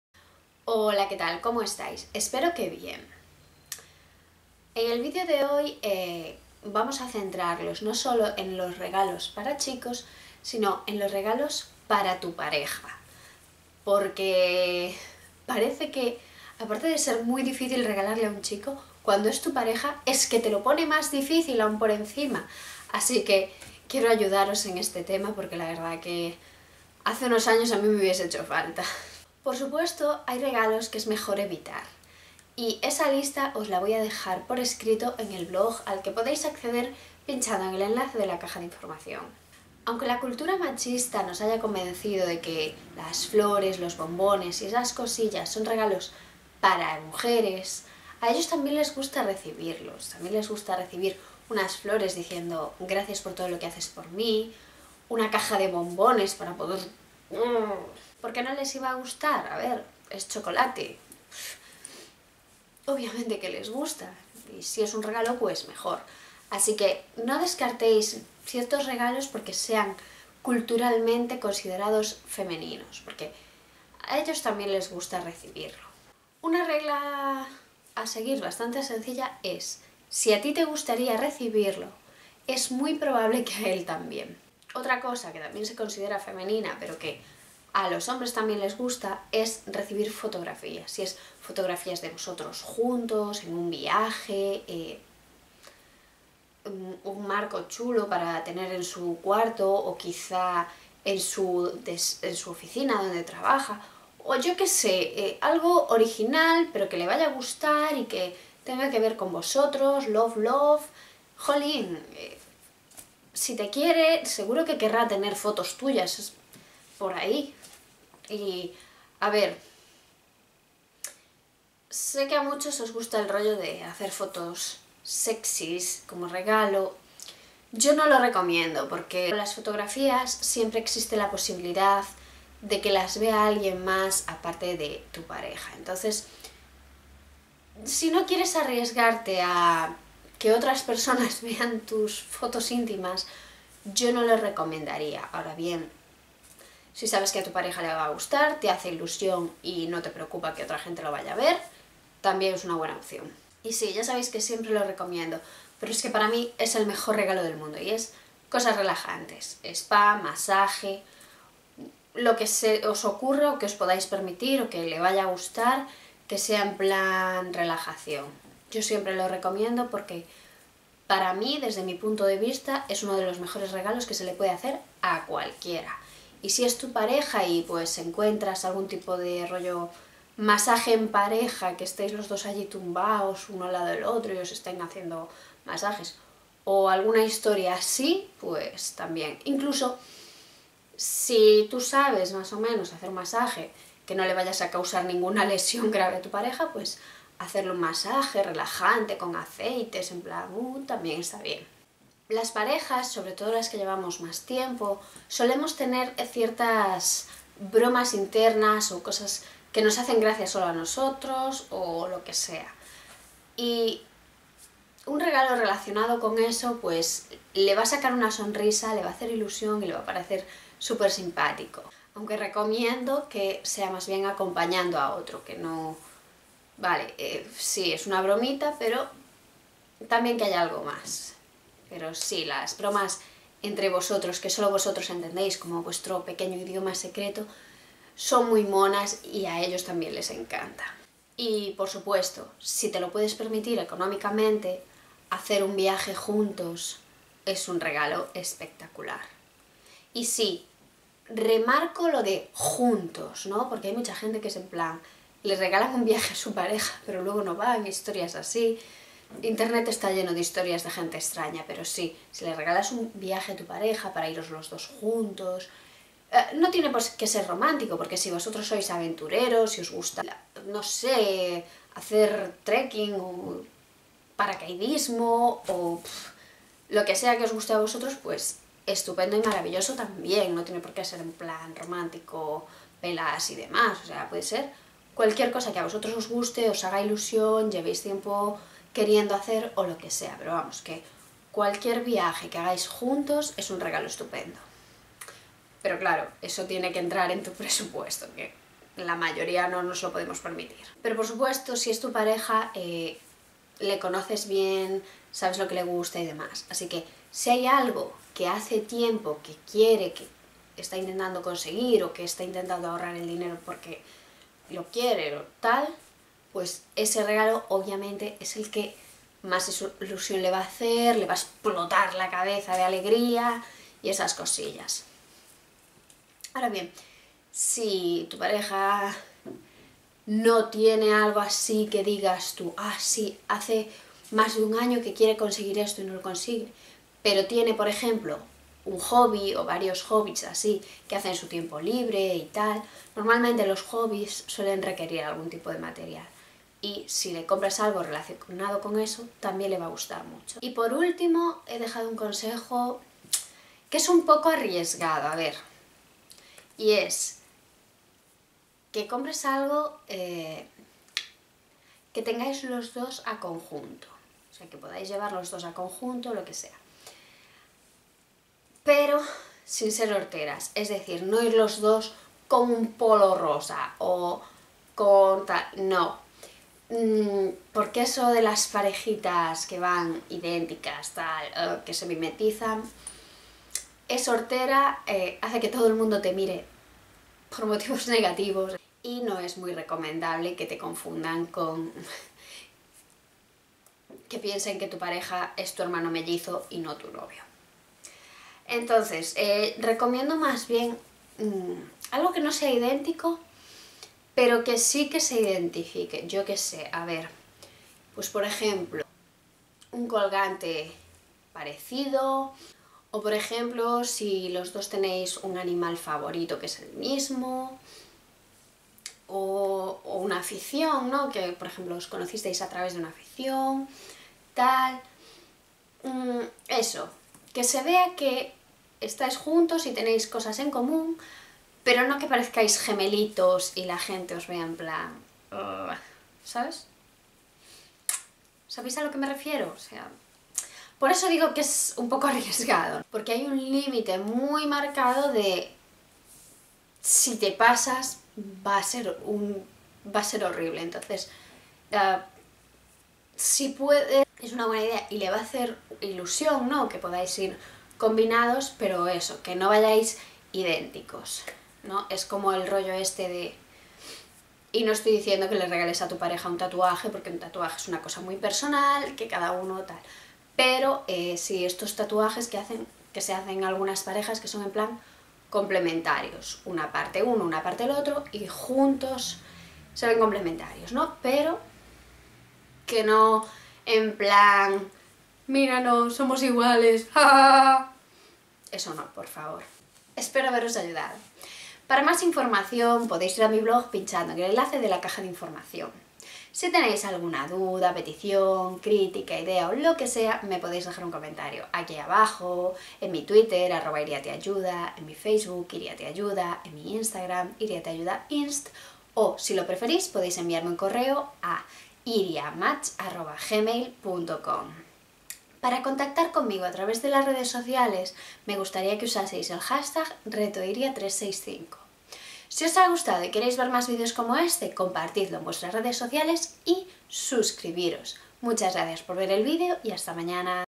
Que regalar a tu novio. Dia 210
No sabes que regalarle a tu novio? No te preocupes nos pasa a todos.